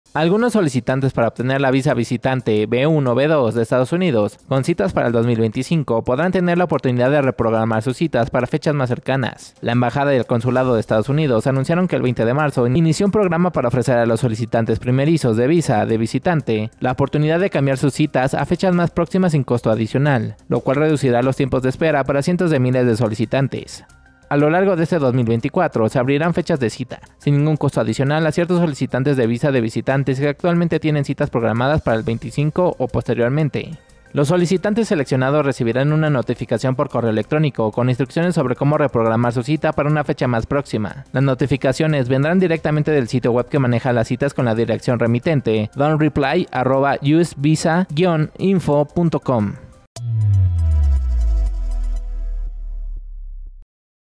Solicitantes para visa de turista de EU podrán adelantar su cita
Solicitantes primerizos para la visa de visitante B1/B2 podrán verse beneficiados con una reprogramación en fechas más próximas.